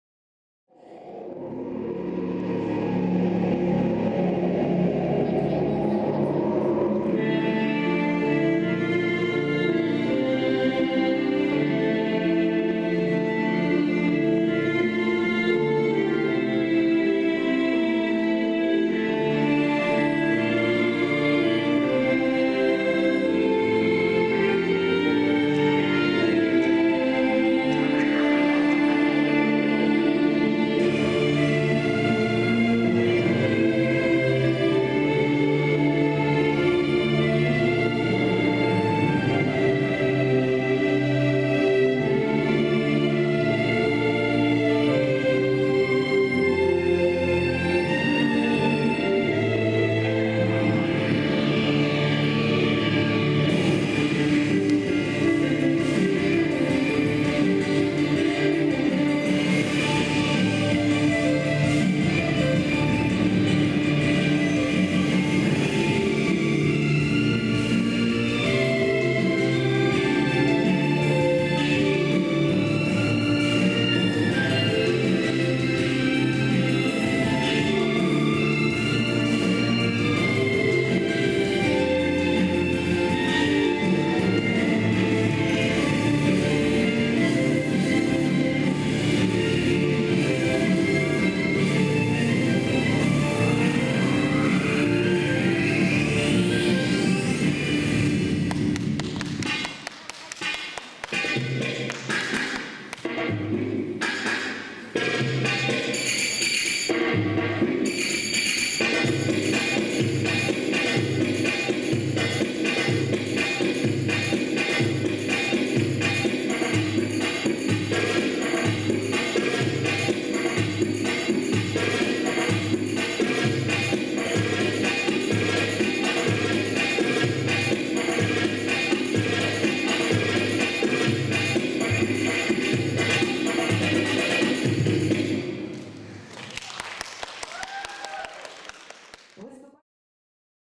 Veronica Tsarfina ⊰⊱ Ukrainian Bellydance Championship '14.
Вероника Царфина на Втором Украинском Чемпионате по восточным танцам  г. Днепропетровск, КДЦ "Днепро Пресс" 19.04.14.
(Дети 2 шоу начинающие) 5,22 .